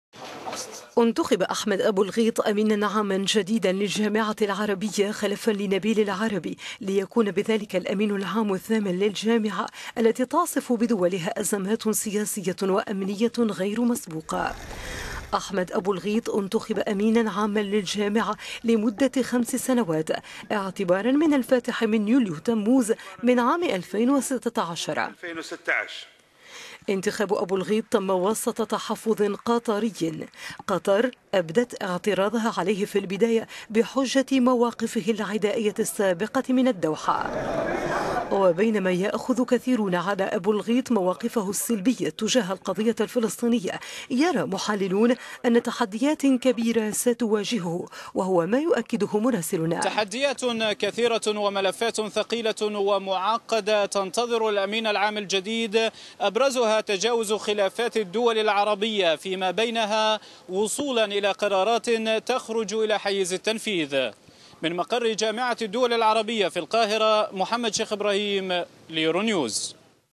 الجامعة العربية تنتخب أحمد أبو الغيط أمينا عاما جديدا
انتخب أحمد أبو الغيط أمينا عاما جديدا للجامعة العربية خلفا لنبيل العربي ليكون بذلك الأمين العام الثامن للجامعة العربية التي تعصف بدولها أزمات سياسية وأمنية غير مسبوقة.

أحمد أبو الغيط انتخب أمينا عاما للجامعة لمدة خمس سنوات اعتبارا من الفاتح من يوليو / تموز من العام 2016 . انتخاب أبو الغيط تم وسط

تحفظ قطري، فقطر أبدت اعتراضها عليه في البداية بحجة "مواقفه العدائية" السابقة ضد الدوحة.

وبينما يأخذ كثيرون على أبو الغيط "مواقفه السلبية" تجاه القضية الفلسطينية، يرى محللون أن تحديات كبيرة ستواجهه  و…